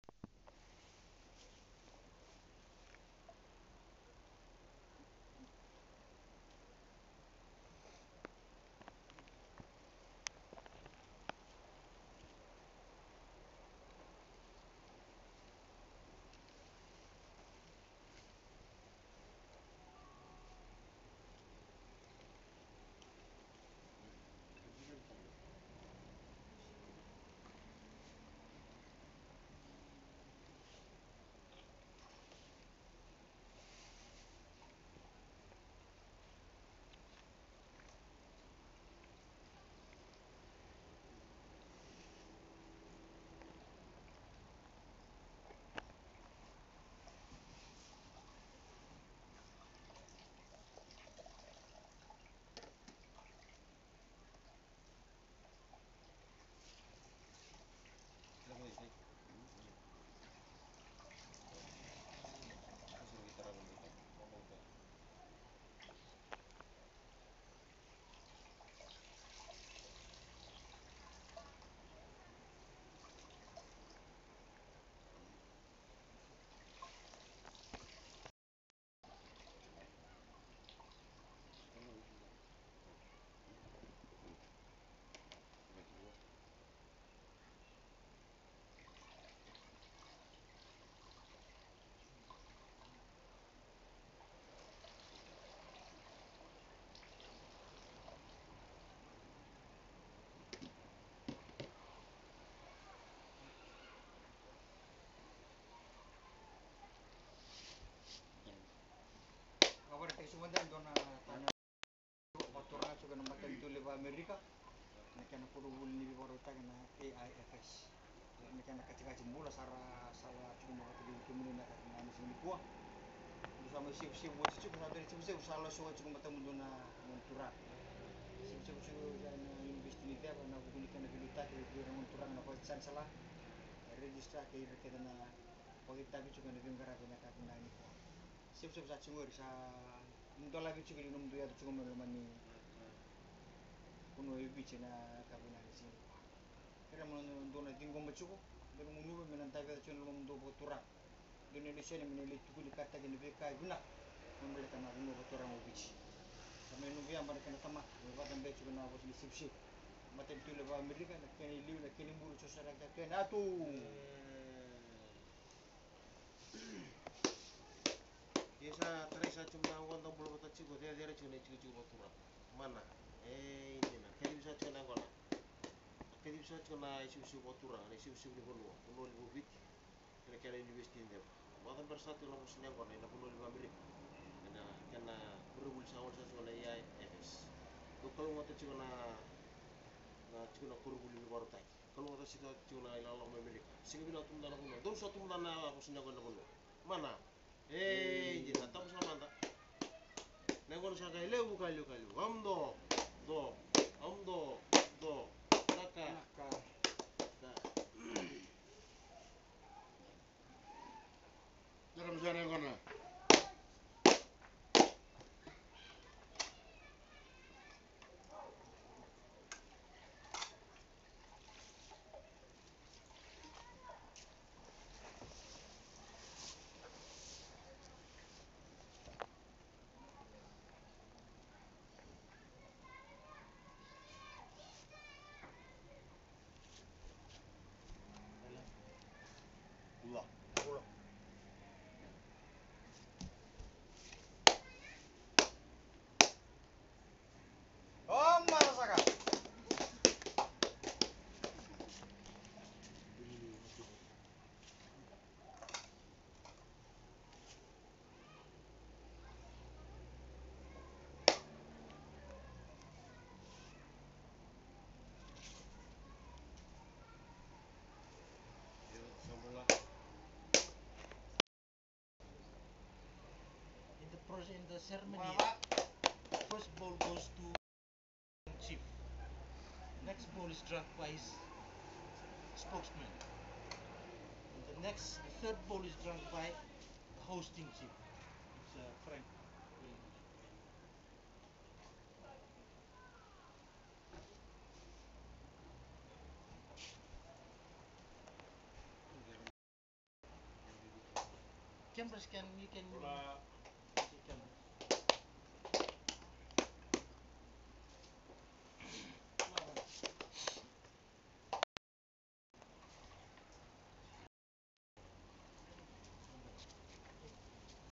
Kava in Fiji - Bula!
It is tradition for guests to be welcomed by the Fiji people with a ceremony in which the drinking of kava takes place. This mud-like liquid was delicious...not, lol, but it was a good experience! Fiji is an amazing and friendly place